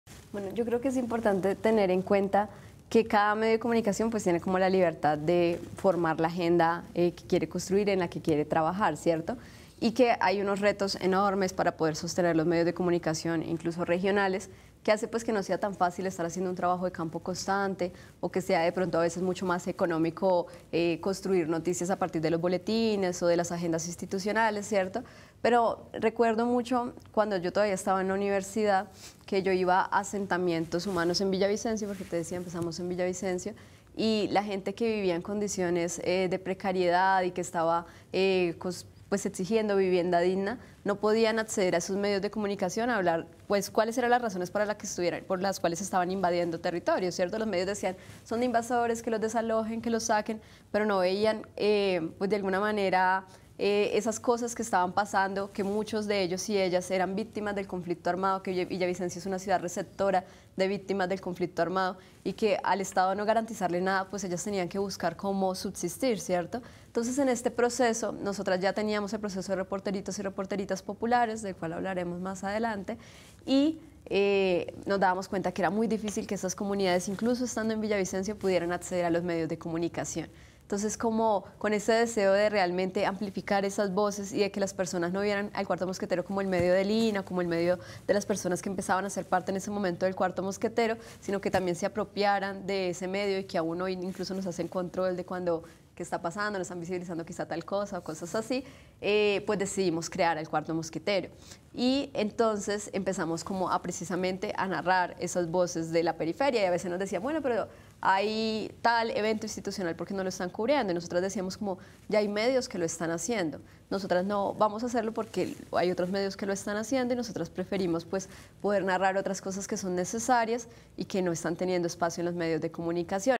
¿Cómo nace 'El Cuarto Mosquetero? | En el Medio - Mesa Capital
MesaCapital | Lina Álvarez, directora de 'El Cuarto Mosquetero', habló EnElMedio sobre qué las llevó a crear un medio de comunicación regional enfocado en el feminismo, la inclusión y los DD.HH. de las comunidades más apartadas de Colombia.